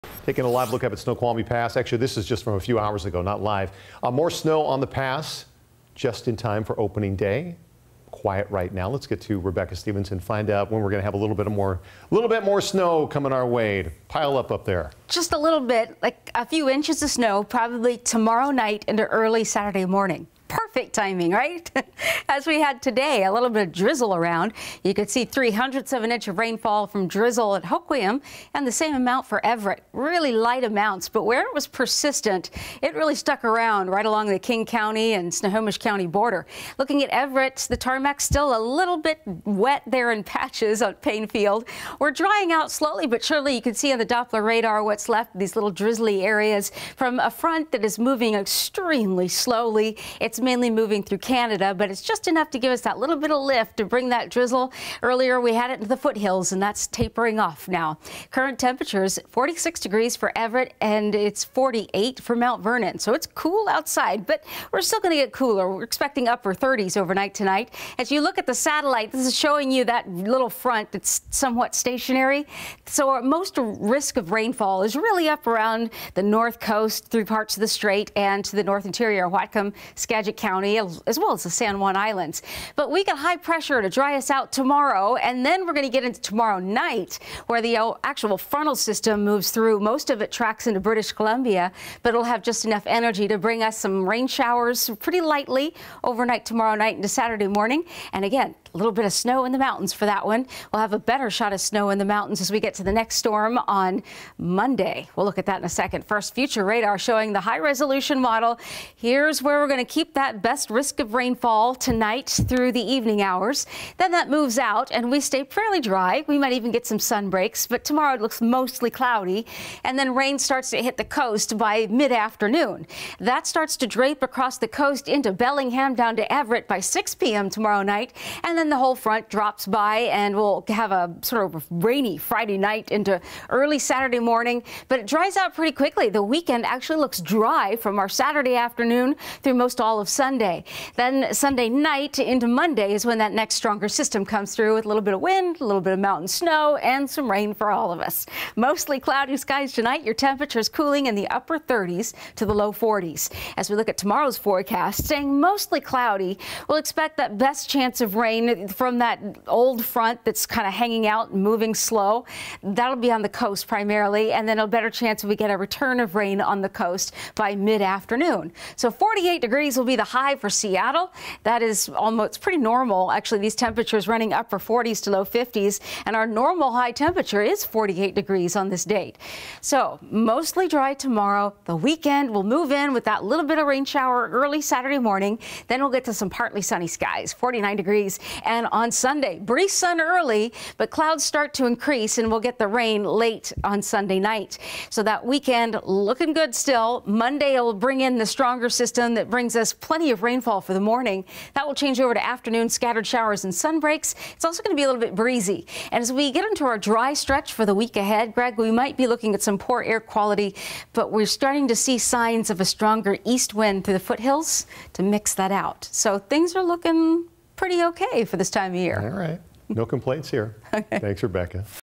KING 5 Weather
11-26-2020 evening forecast with KING 5 Meteorologist Rebecca Stevenson.